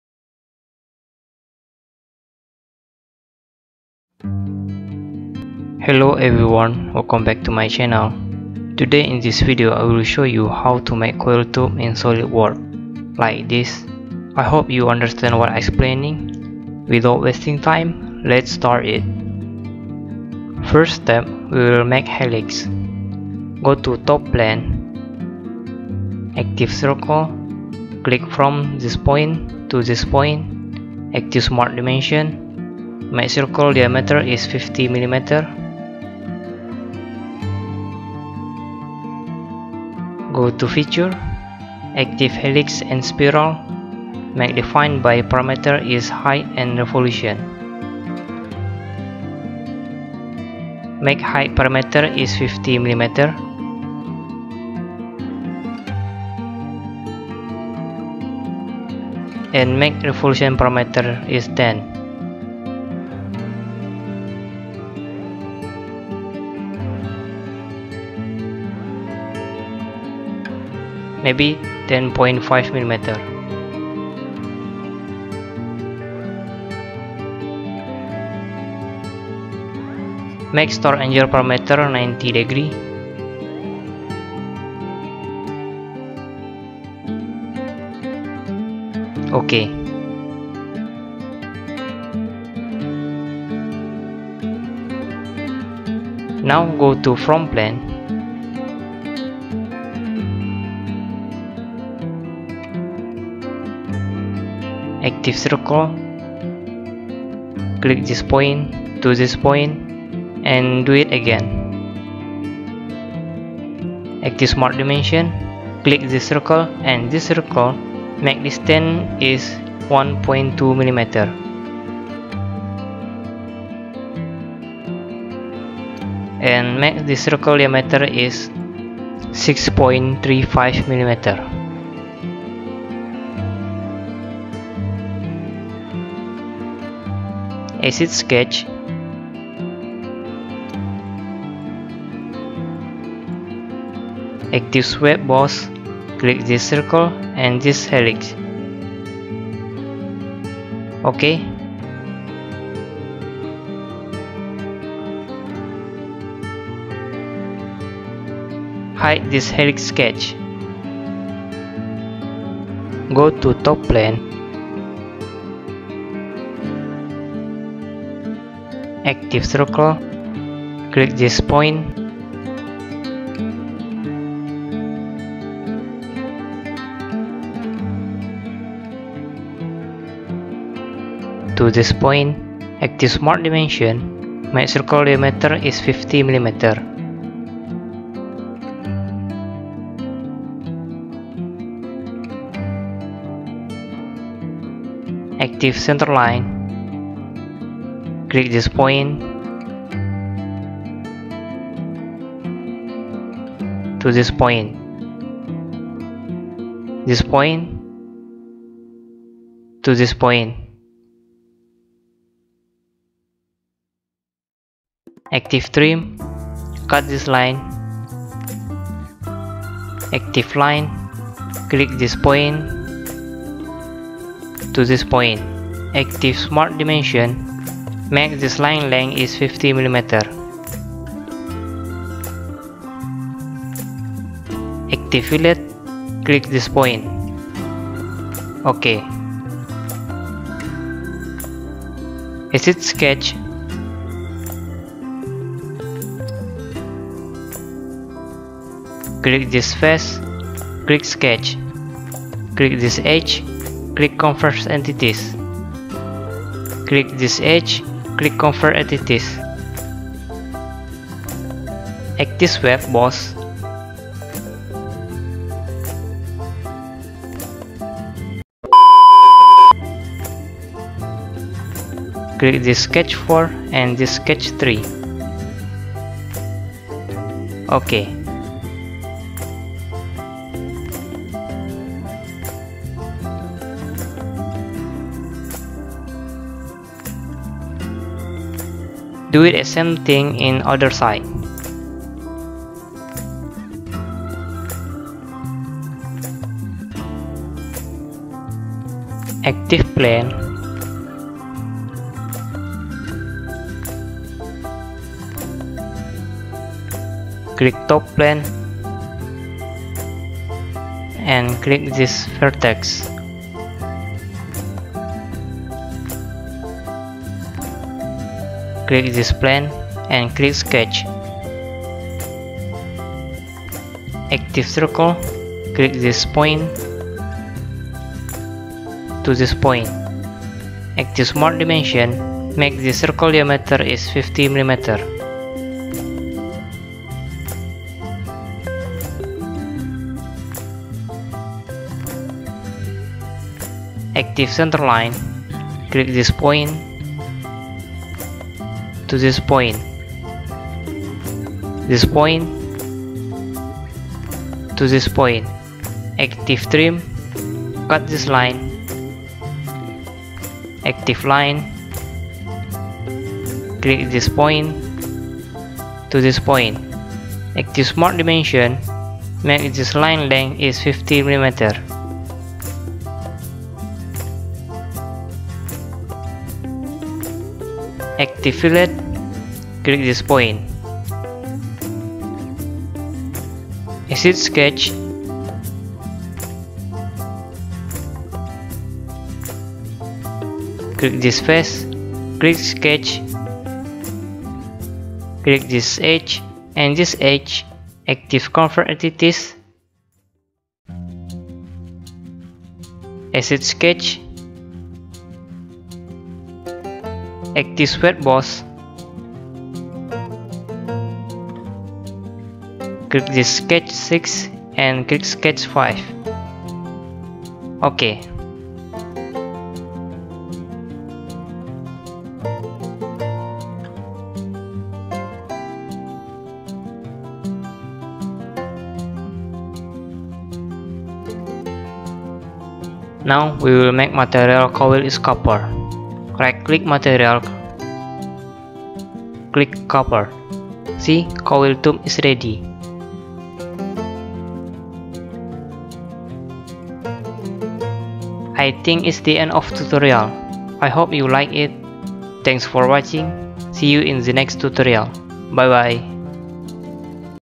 Design a Coil Copper Tube use Solidworks
In this video i show you how to Design a Coil Copper Tube in Solidworks.
I hope this video will help you to design use solidworks
Just enjoy it, and like it.